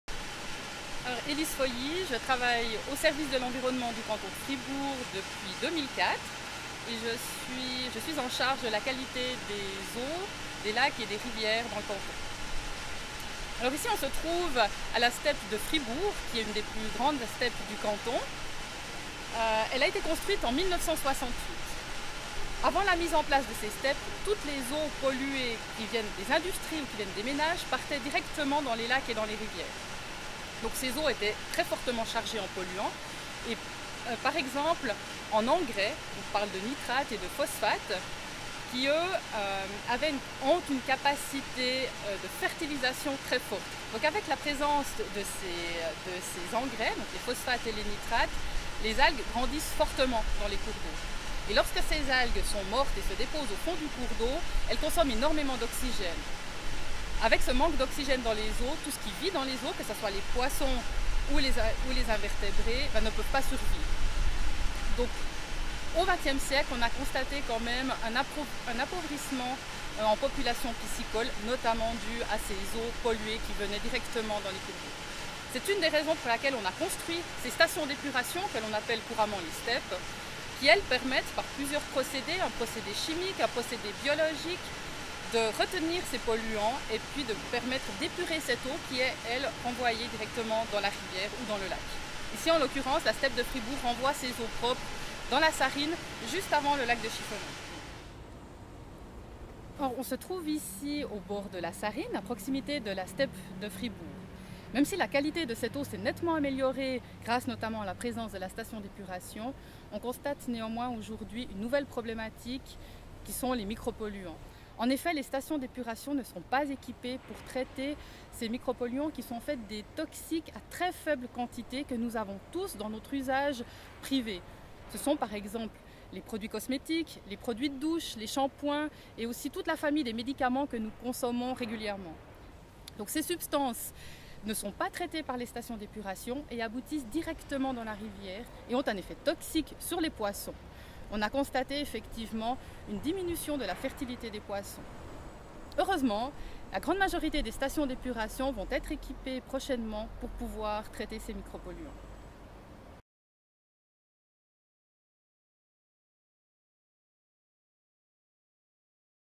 Au Fil de la Sarine: la qualité des lacs et des rivières
Au Fil de la Sarine: la qualité des lacs et des rivières. Sensibilisation aux risques de pollution. Dès les années 1960, l’implantation progressive des STEP a permis une amélioration globale de la qualité des eaux de nos rivières. Explications d'Elise Folly, cheffe du secteur eaux superficielles au Service de l'environnement